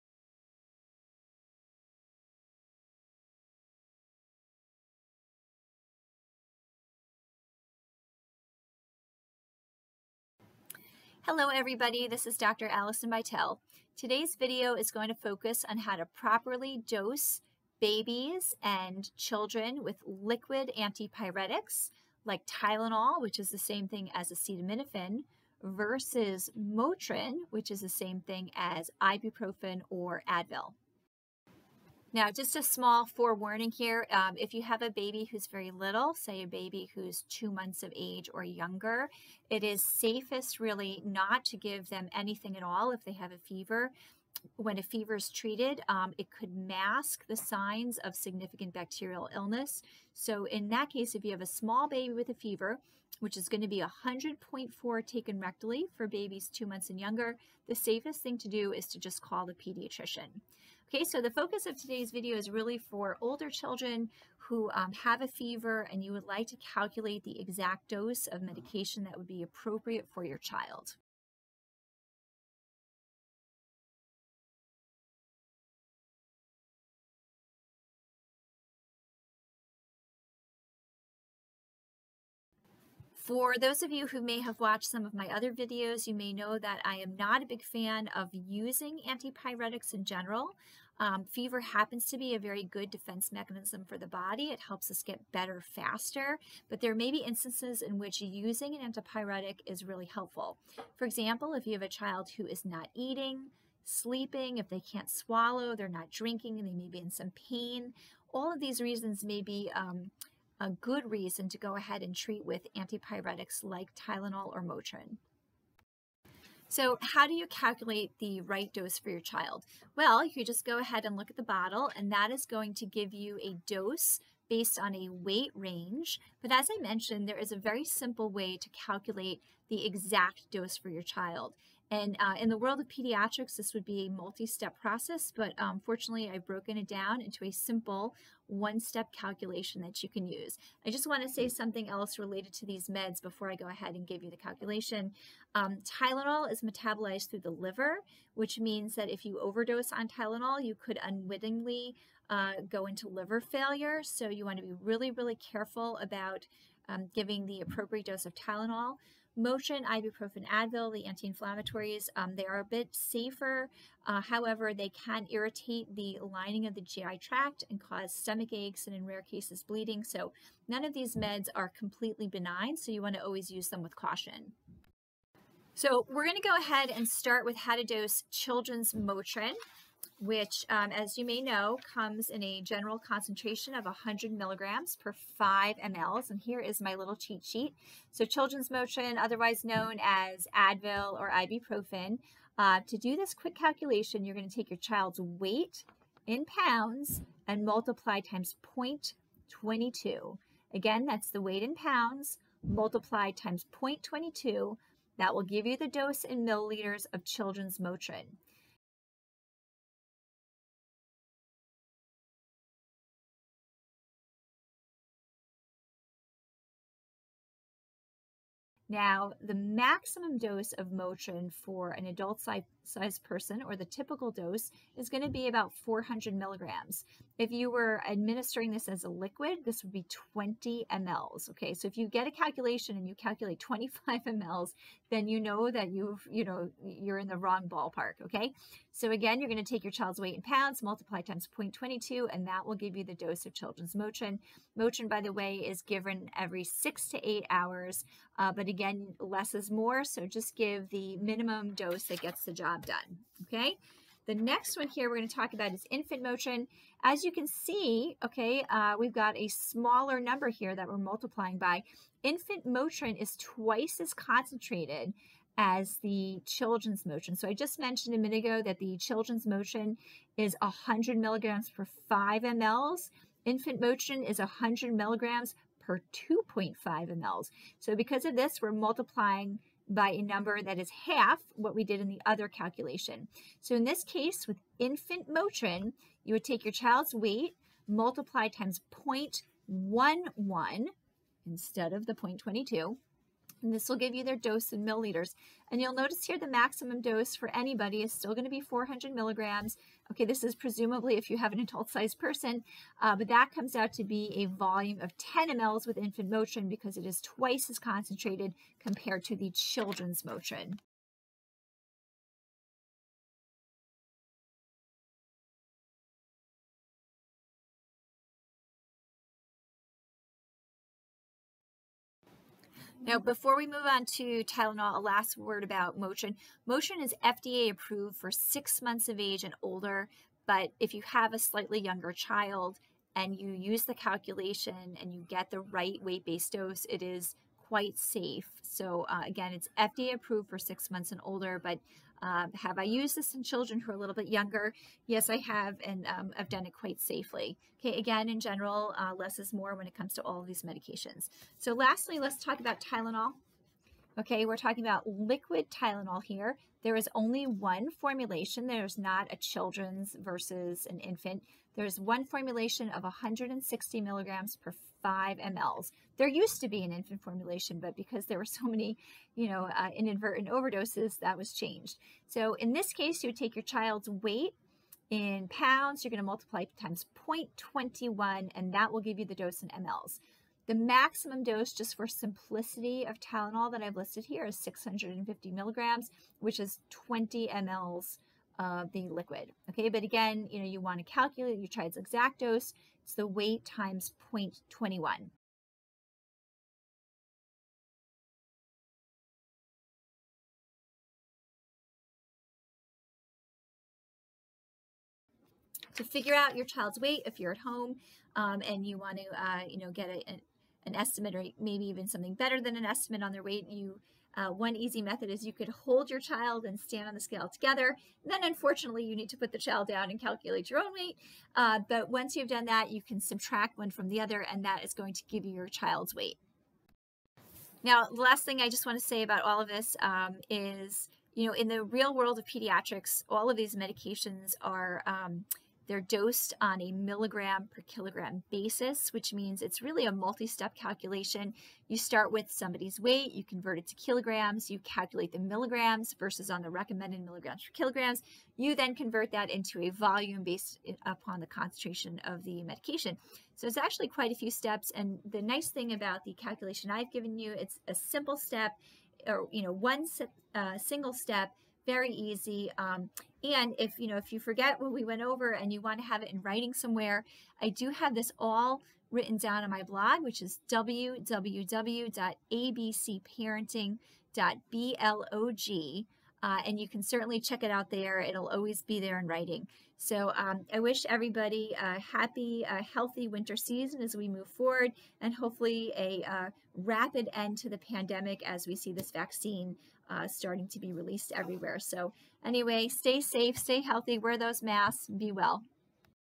How to dose liquid Tylenol or Motrin for infants and children
This video demonstrates a simple one-step calculation to determine the correct dose of liquid Tylenol or Motrin for babies and children. The calculation is explained at 3:00.